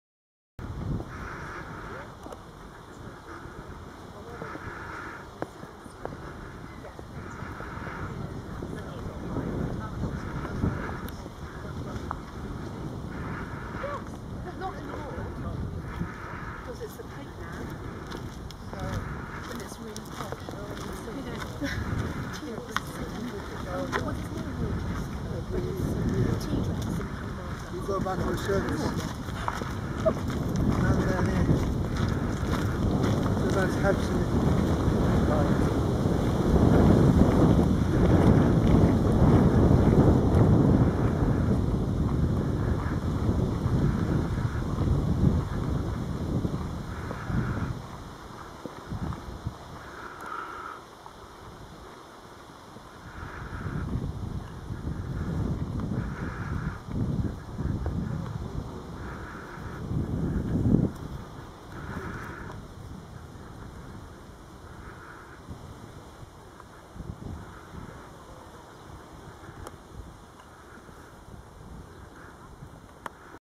Treat her Very Special @ the weekend -The Pig on the Beach Hotel
Does your girl deserve a special weekend by the sea ? Then this could tick or the boxes ...The Pig on the Beach Luxury Hotel at Studland Bay Dorset .